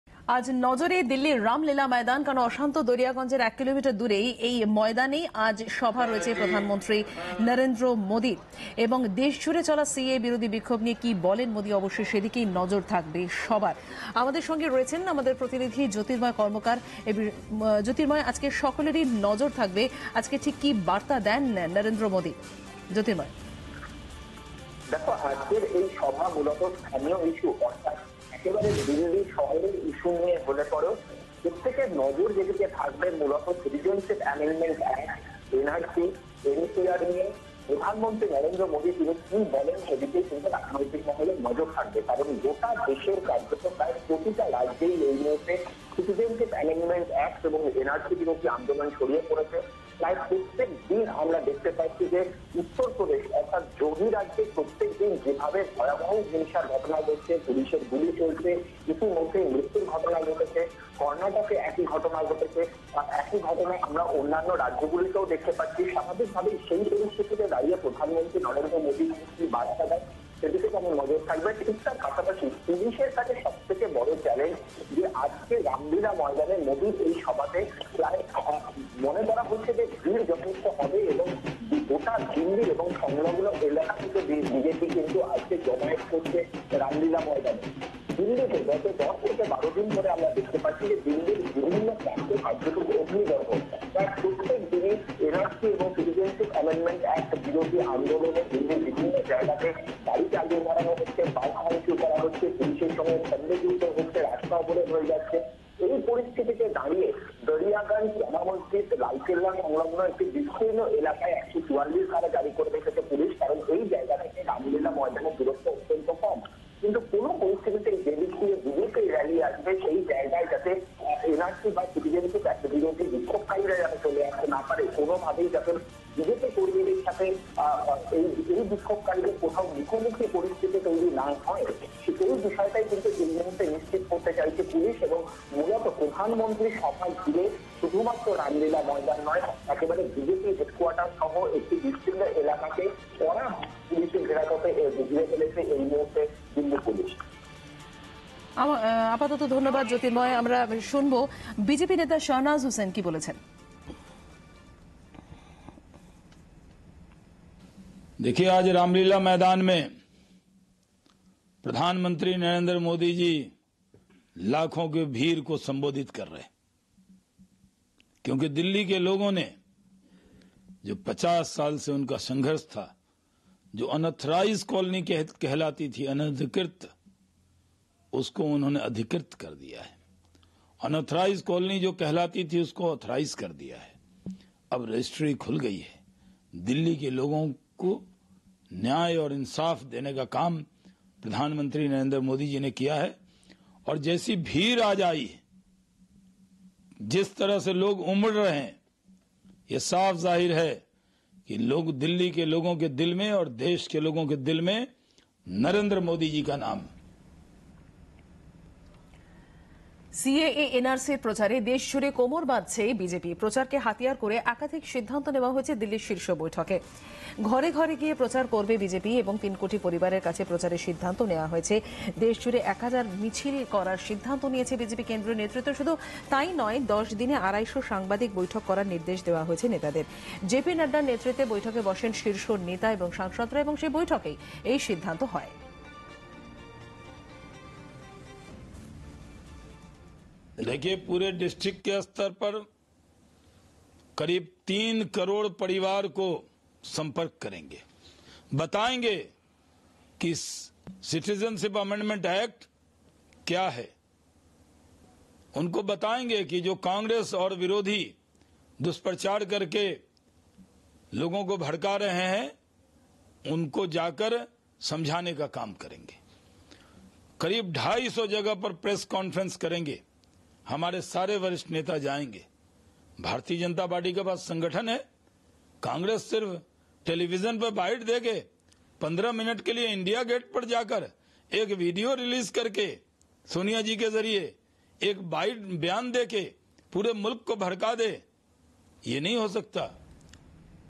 Modi's campaign not far away from Dariyaganj
অশান্ত দরিয়াগঞ্জের অদূরে মোদীর সভা